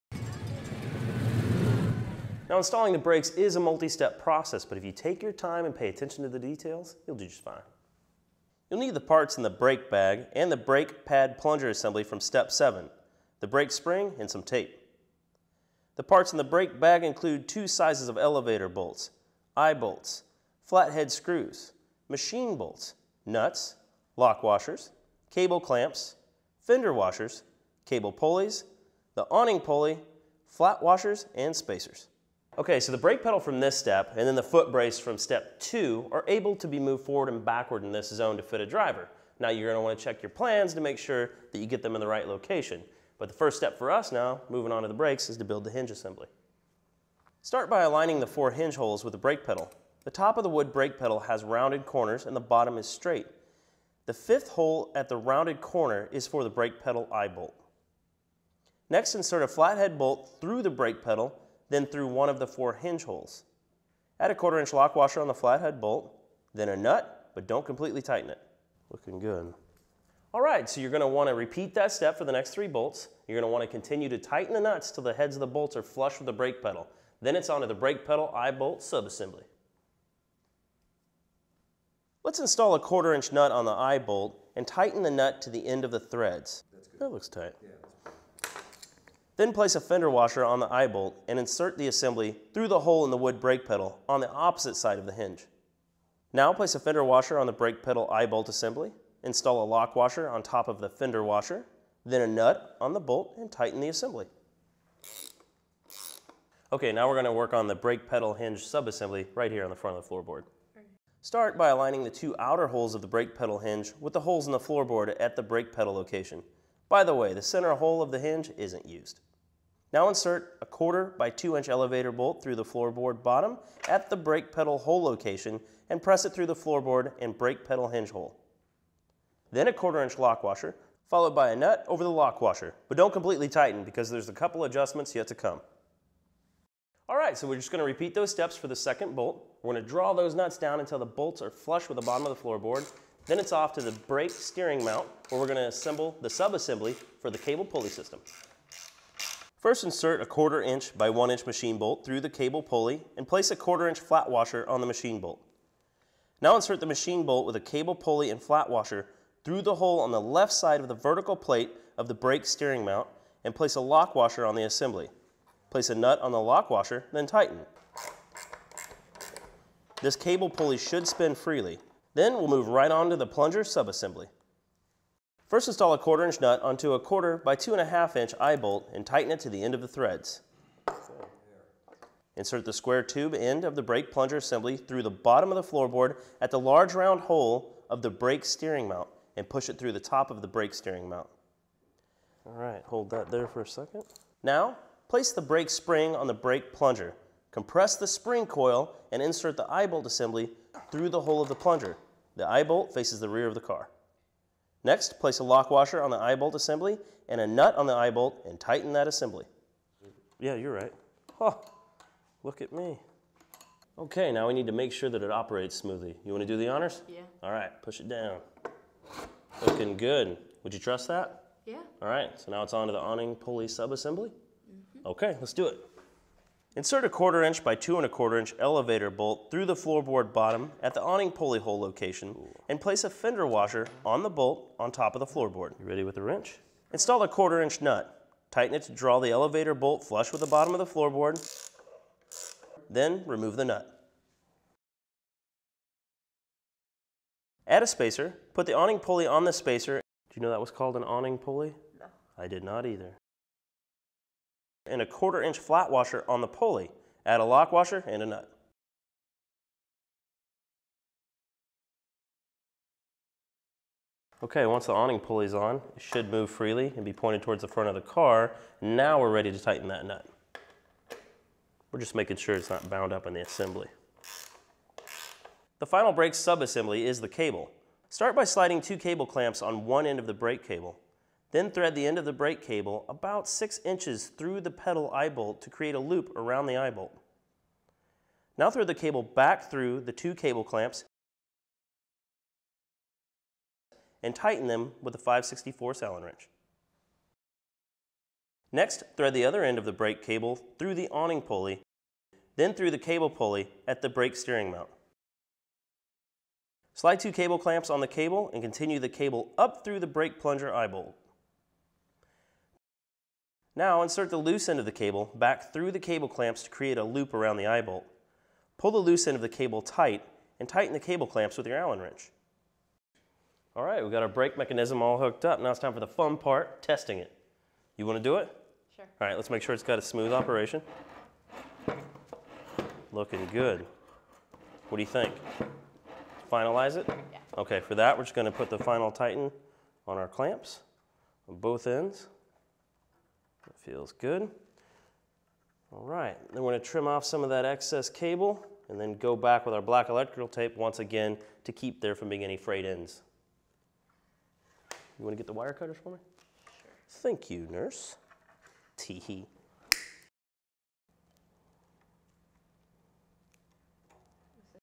Super Stock Car - Step 8 - Brake Installation
Step 8 for building a Soap Box Derby® Super Stock car.